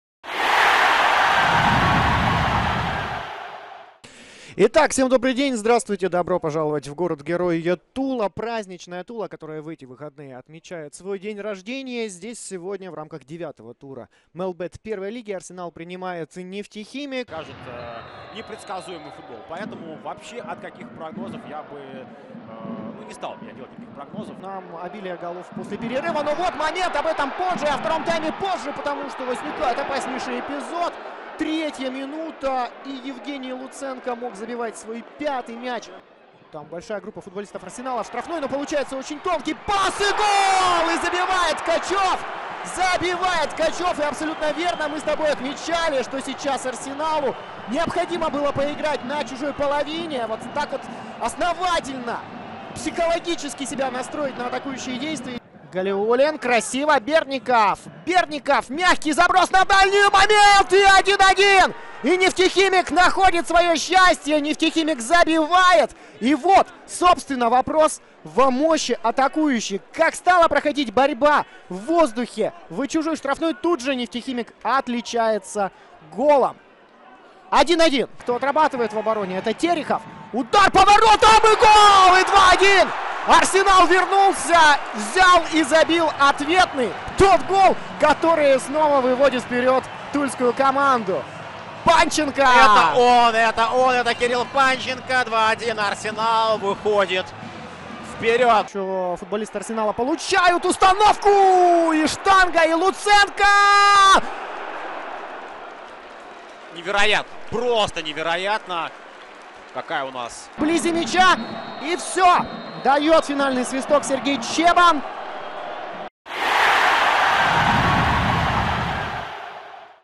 120 секунд о матче «Арсенал» — «Нефтехимик» | 9 тур МЕЛБЕТ-Первой лиги
Лучшие моменты из игры «Арсенала» (Тула) и «Нефтехимика» (Нижнекамск) 11 сентября 2022.